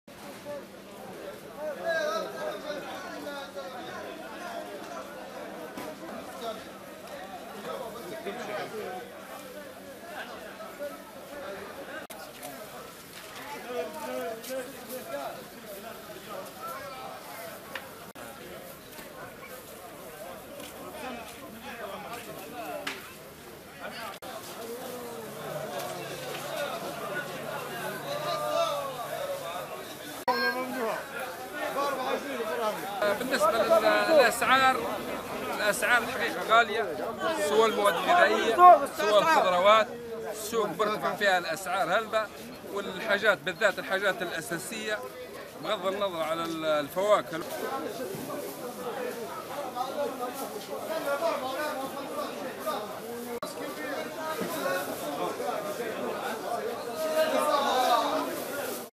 الليبيون يقبلون على التسوق لرمضان رغم الشكوى من ارتفاع الأسعار
تشهد الأسواق في طرابلس إقبالاً كبيراً من الليبيين مع بداية شهر رمضان على رغم تذمر الكثيرين من ارتفاع الاسعار.تقرير بالصوت الطبيعي